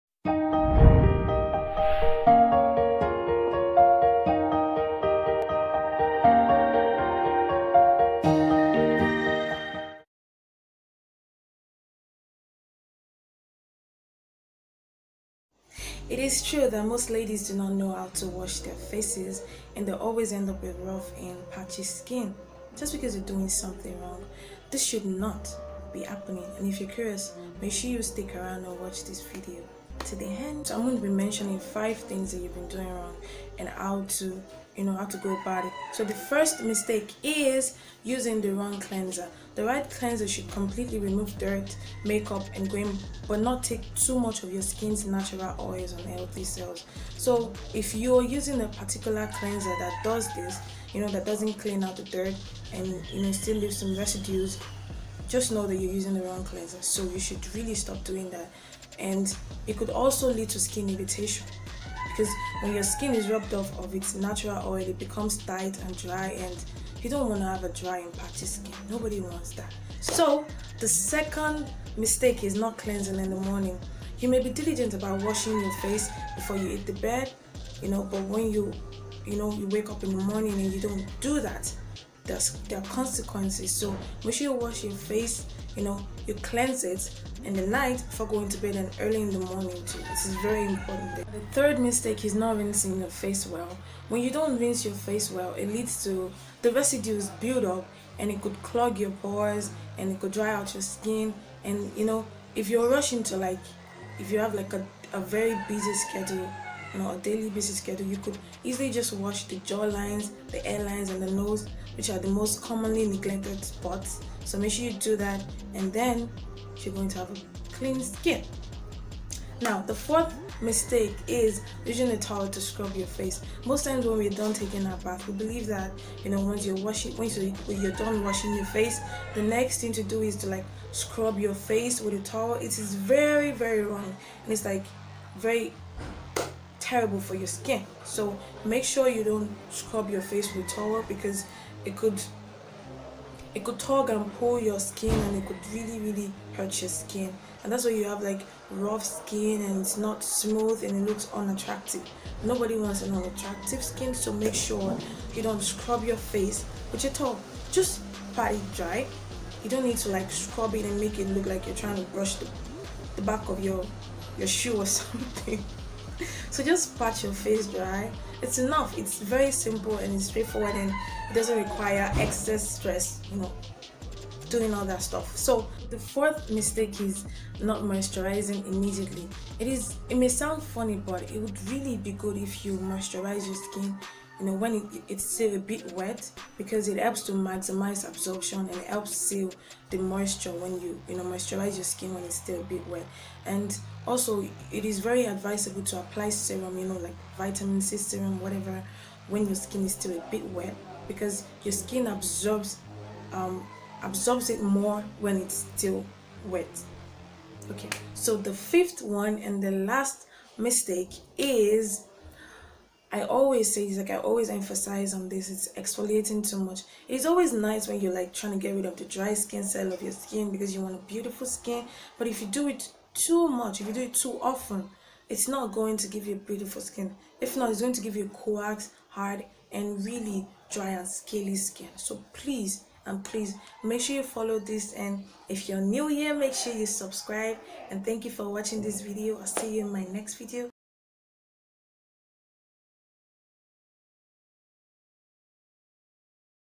THINGS YOU SHOULDN'T BE DOING WHILE WASHING YOUR FACE//CAUSES OF PATCHY AMD FLAKY SKIN
The reason why some people have rough and patchy skin is because they are not washing their faces right , here's the list of things people do wrong while washing their faces .